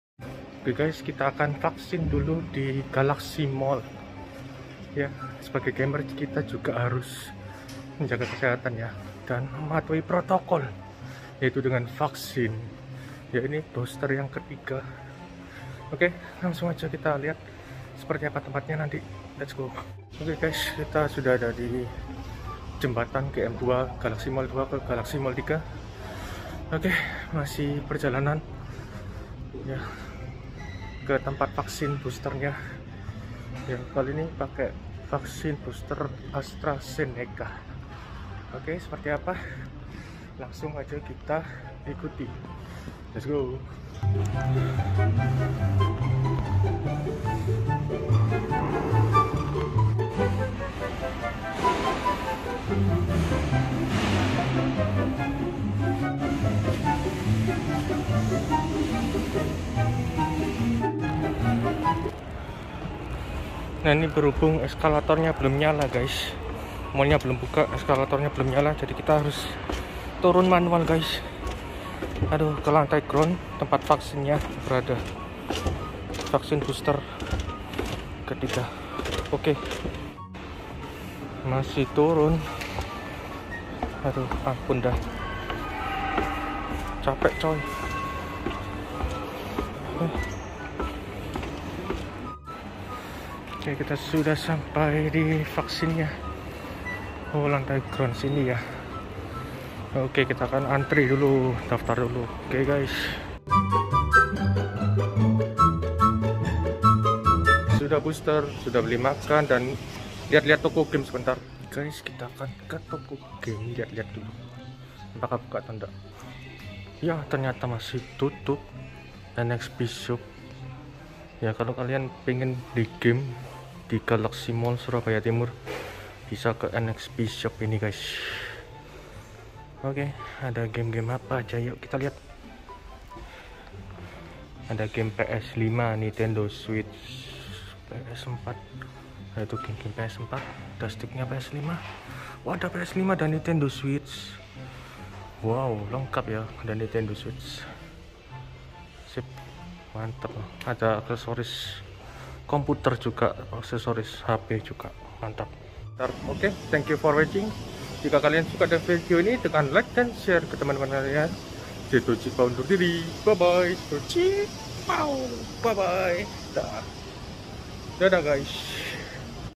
Vaksin ke 3 Booster Astrazaneca, dan intip Toko Game di Galaxy Mall Surabaya
vlog weekend vaksin ke 3 booster astra zaneca sekaligus kita akan meliha toko game di surabaya timur.

Dukung terus yaa guysssss..

Thanks Guys and ENJOYYYYYYYY.